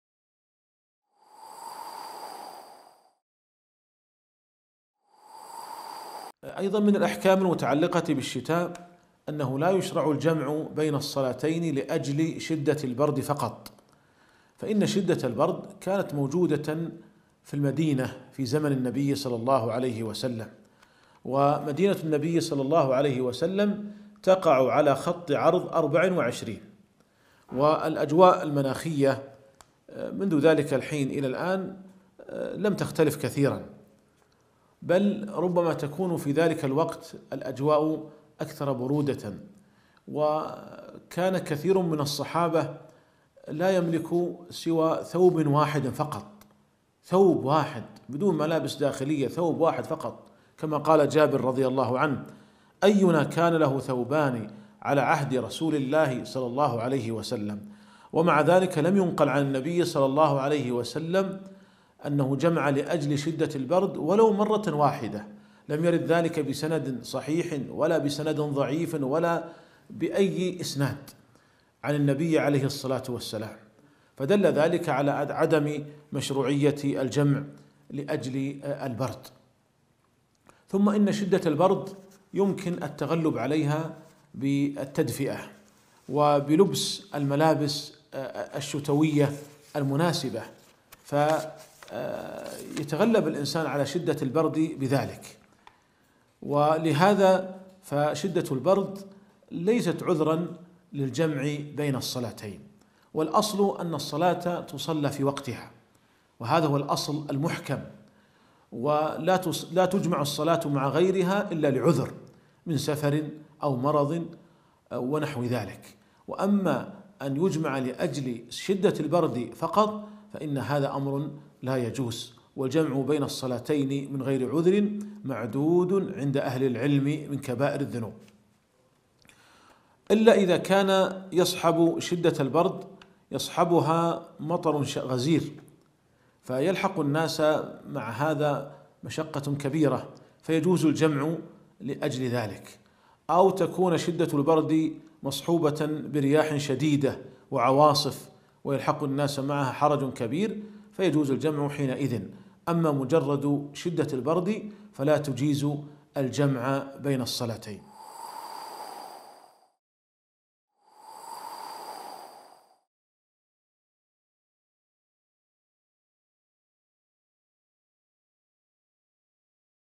268 | ( لايجمع بين الصلاتين لشدة البرد فقط 6-12 ) ـ لفضيلة الشيخ أ.د.سعد الخثلان
مقتبس من محاضرة بعنوان : ( أحكام_الشتاء ) الثلاثاء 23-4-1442
بعنوان :  ( لا يجمع بين الصلاتين لشدة البرد فقط 6-12 )
 لفضيلة الشيخ أ.د.سعد بن تركي الخثلان